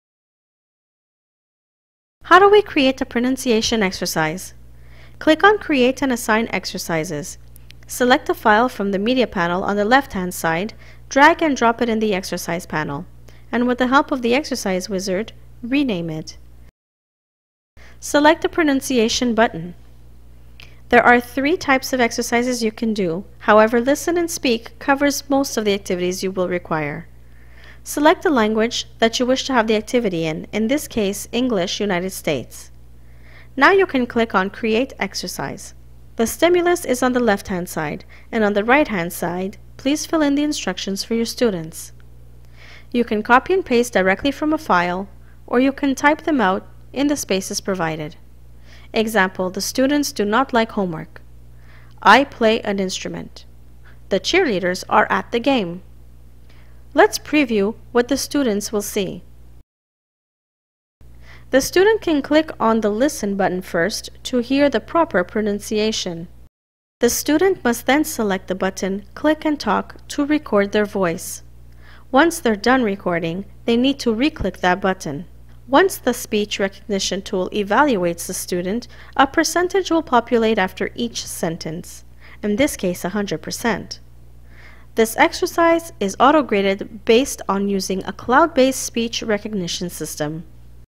14 SC+ Platform: Pronunciation Exercises
Video How to on creating a pronunciation exercise using the SmartClass+ Platform.